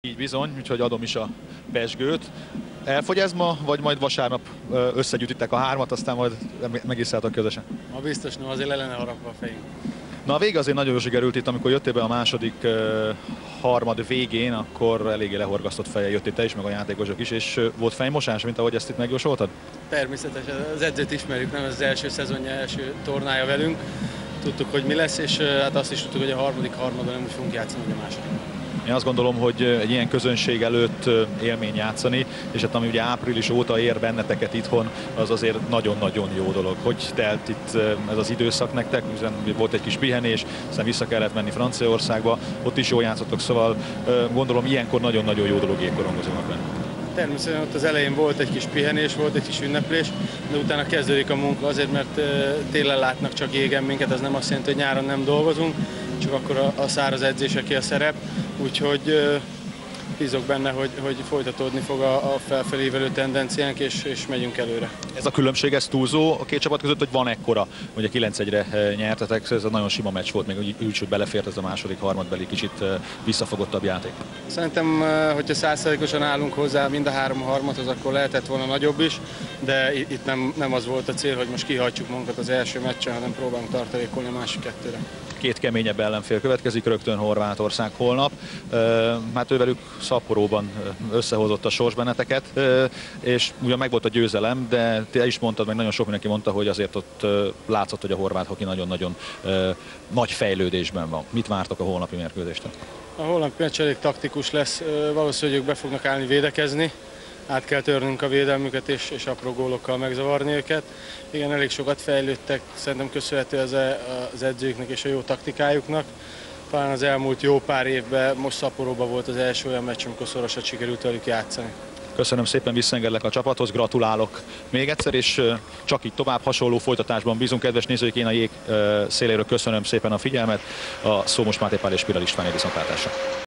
Interjú Ladányi Balázzsal
Interjú Ladányi Balázzsal a Magyarország - Szerbia Olimpiai Selejtező jégkorong mérkőzés után. 2008. November 7.